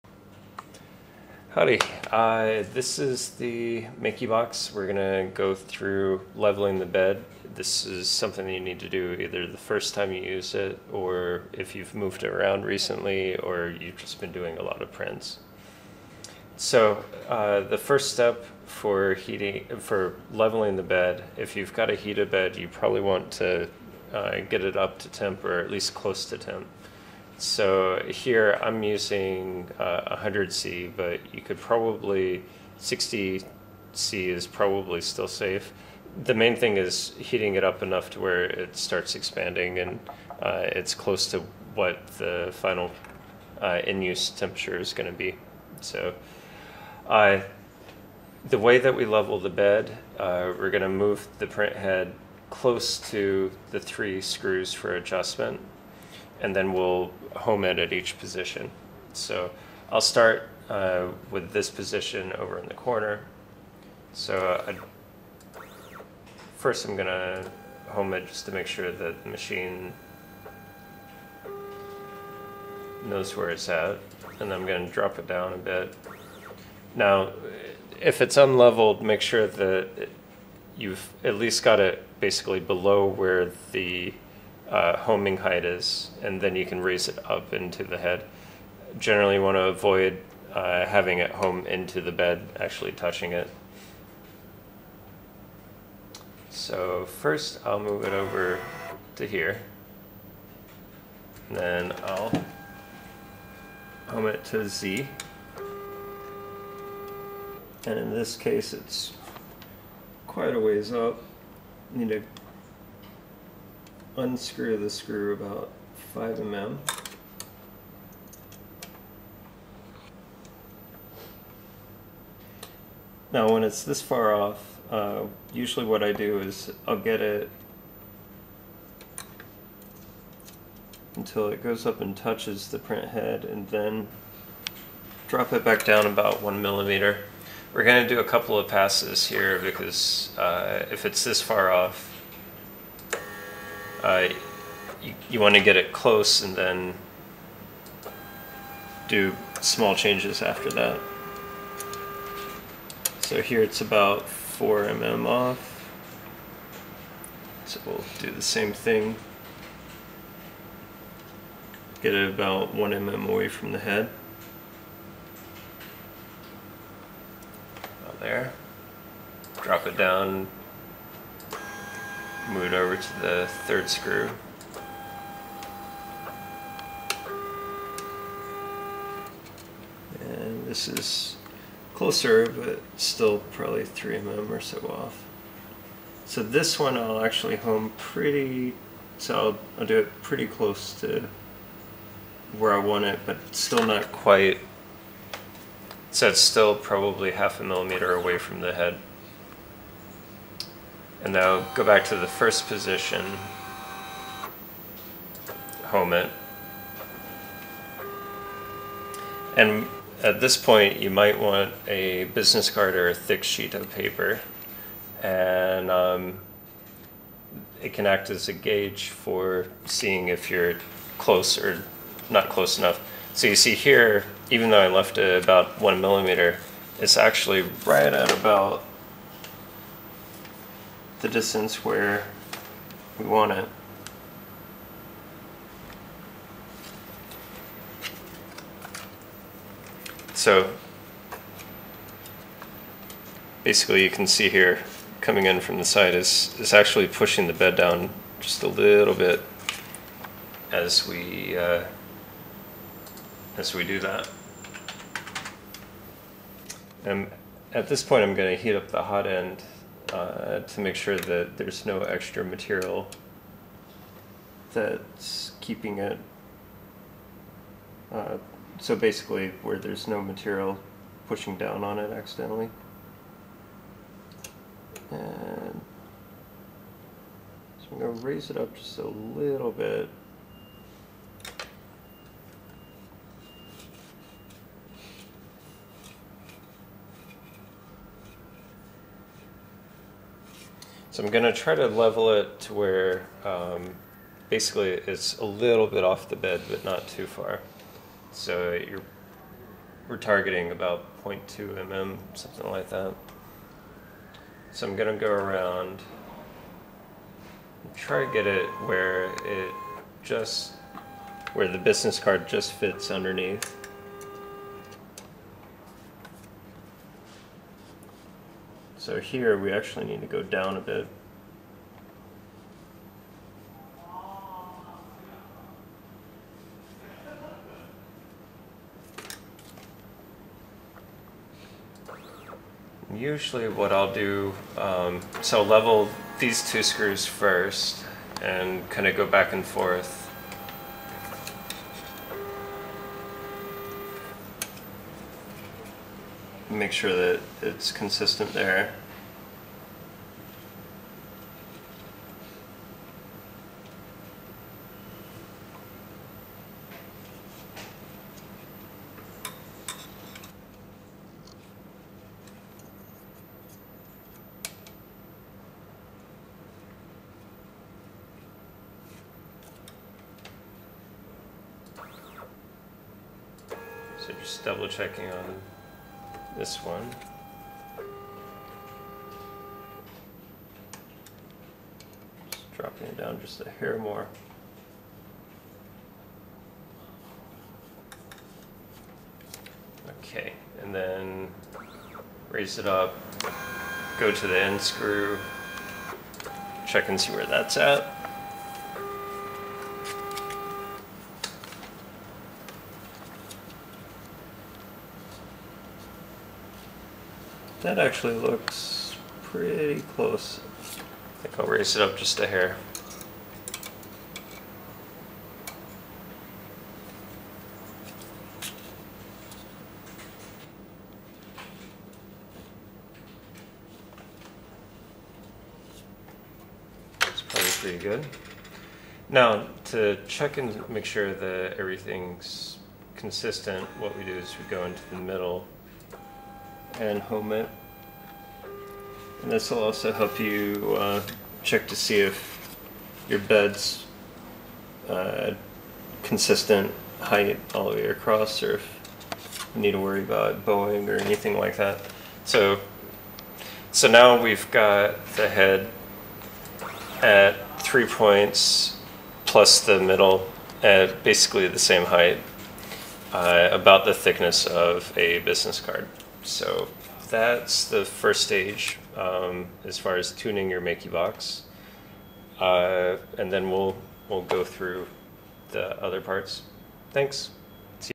MakiBox - How to level your print bed.
This is a guide for how to level your print bed before printing or after you move your machine. You will need to do this periodically or before your first print.

We are showing the heated stainless print bed from the MakiBox A6 HT, but the procedure is the same for the LT bed.